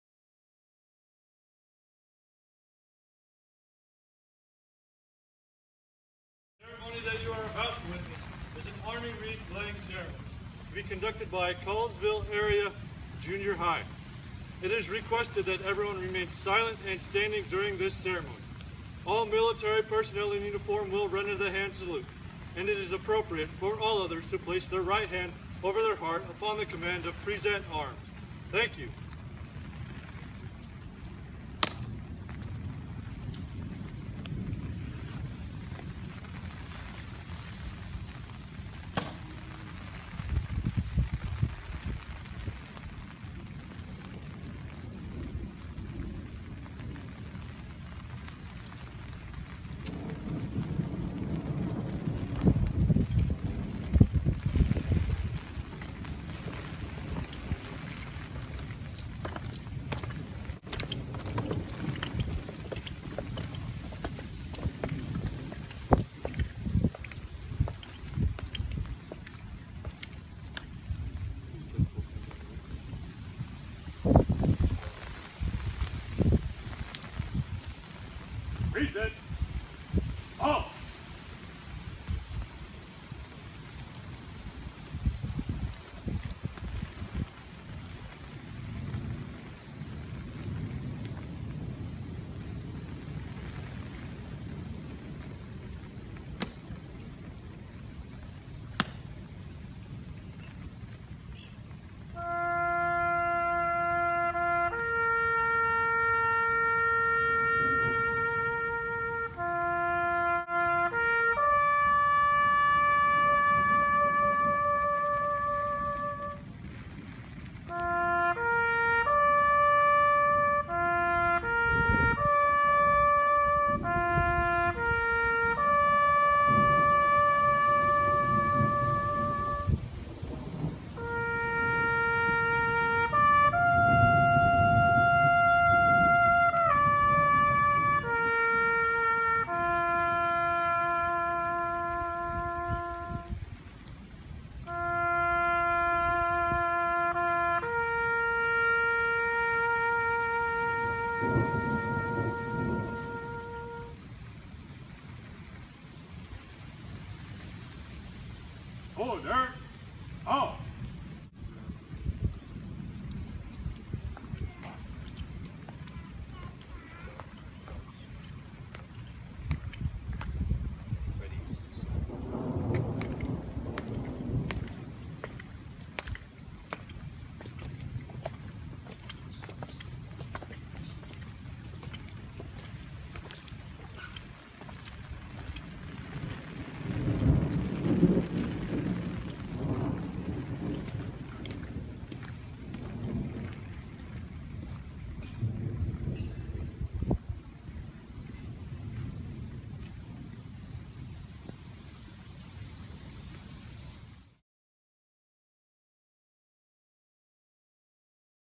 CAJH Students laying a Wreath at the Tomb of the Unknown
Four Connellsville Junior HIgh students lay a Wreath at the Tomb of the Unknown Soldier to honor those who fought to protect this country and payed the ultimate price for it.